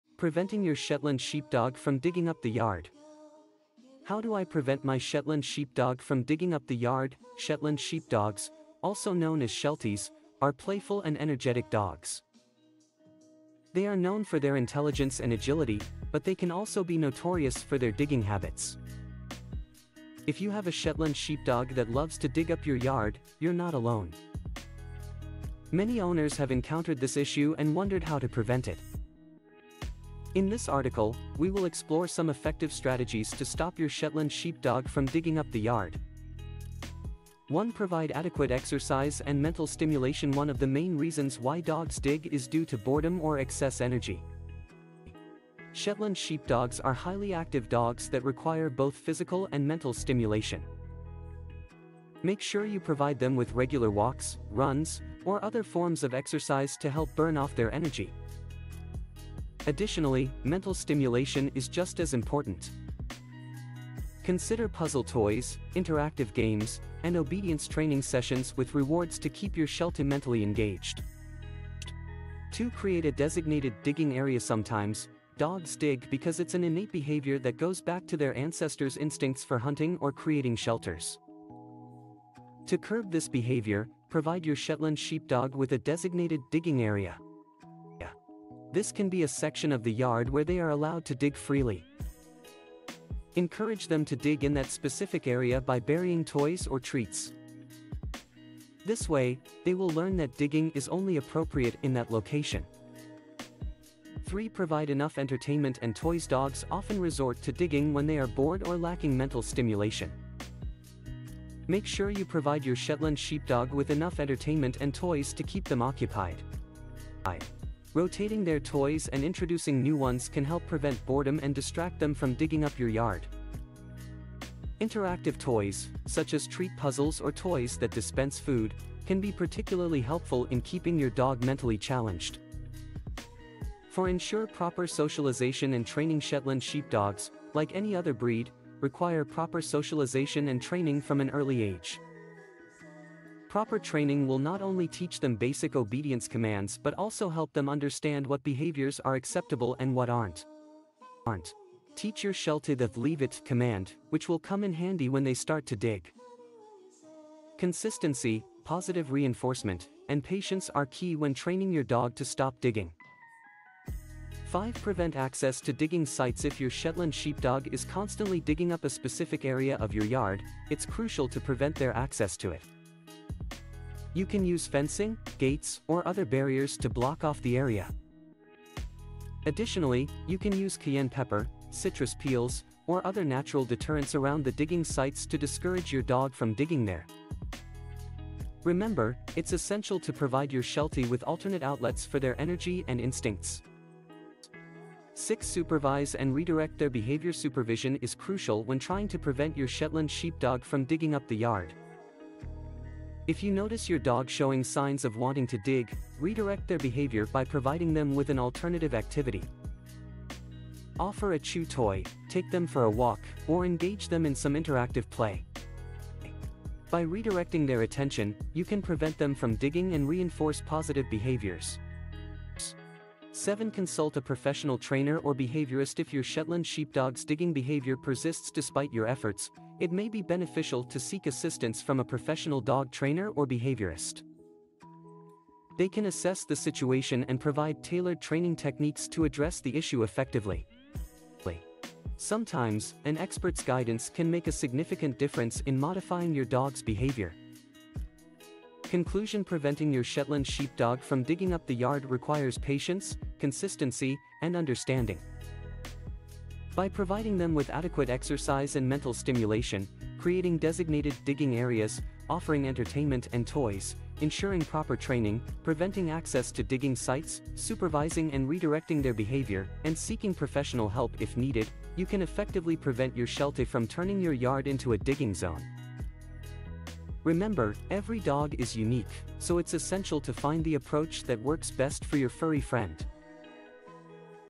Preventing Your Shetland Sheepdog From Digging Up the Yard
How do I prevent my Shetland Sheepdog from digging up the yard?

Shetland Sheepdogs, also known as Shelties, are playful and energetic dogs  They are known for their intelligence and agility, but they can also be notorious for their digging habits  If you have a Shetland Sheepdog that loves to dig up your yard, you're not alone  Many owners have encountered this issue and wondered how to prevent it  In this article, we will explore some effective strategies to stop your Shetland Sheepdog from digging up the yard
00:00 "Preventing Your Shetland Sheepdog From Digging Up the Yard"